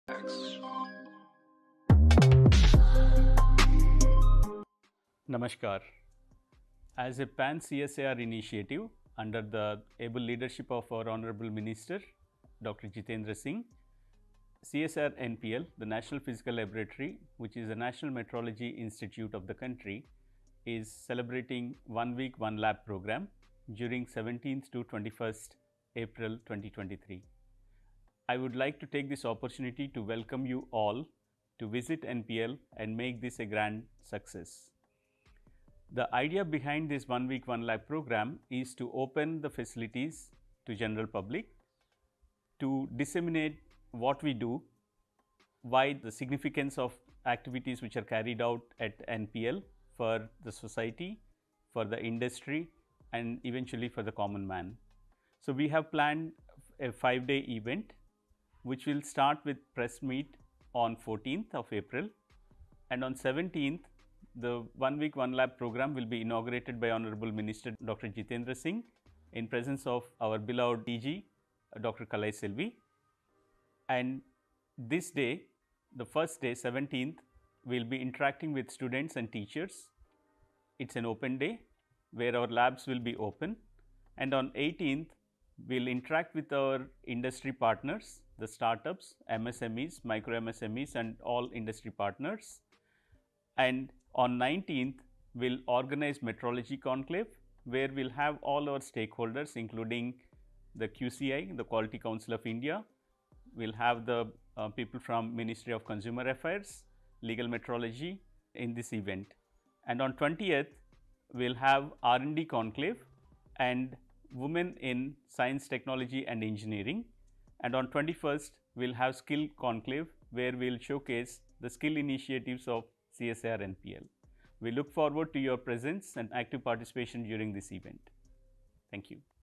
CSIR-NPL One Week One Lab Campaign
Invitation from Prof. Venugopal Achanta, Director CSIR-NPL